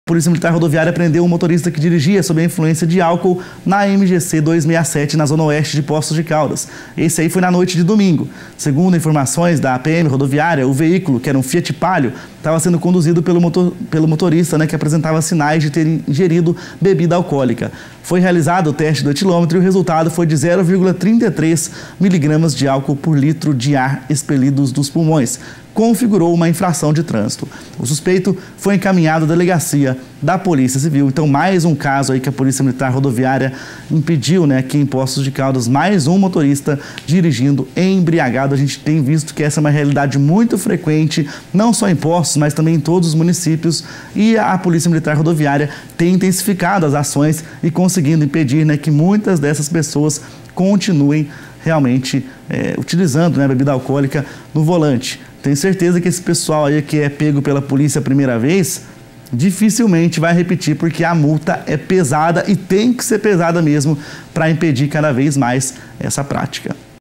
HOMEM É PRESO POR DIRIGIR EMBRIAGADO NA MCG-267
Foi realizado o teste de alcoolemia com resultado de 0,33 miligramas de álcool por litros de ar expelido dos pulmões, configurando infração de trânsito.